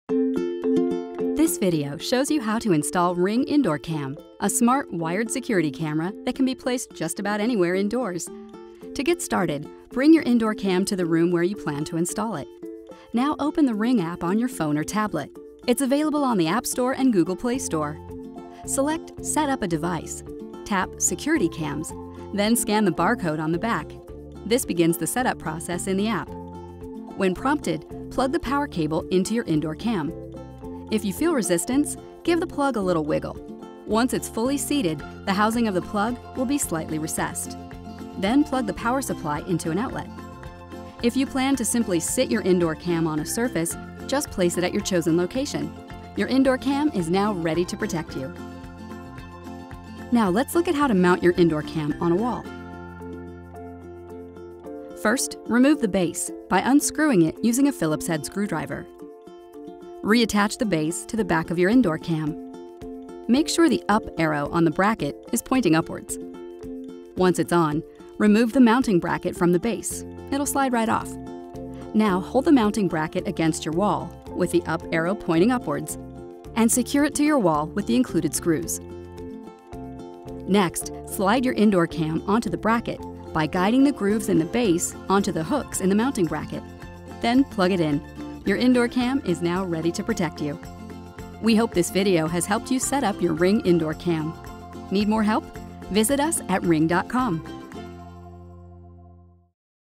How to Install Ring Indoor Cam
Learn how to install Ring Indoor Cam, a smart wired security cam that goes just about anywhere indoors.